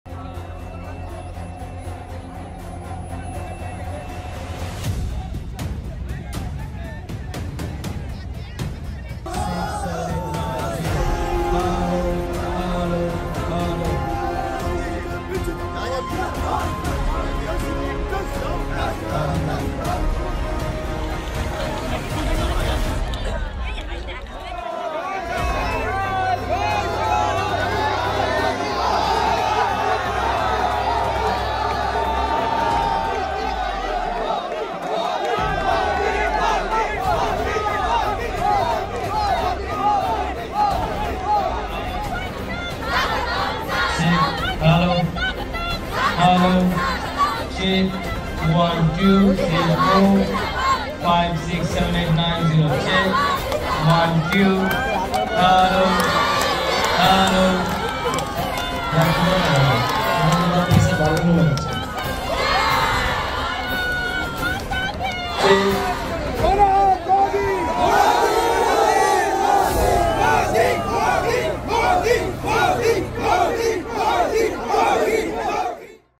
আরামবাগের জনতা, দেখাচ্ছে ক্ষমতা!
আর কিছু সময় পরেই এই স্থানে আসবেন প্রধানমন্ত্রী। তার বক্তব্যের সঙ্গে করবেন গোটা পশ্চিমবঙ্গবাসীর মন জয়।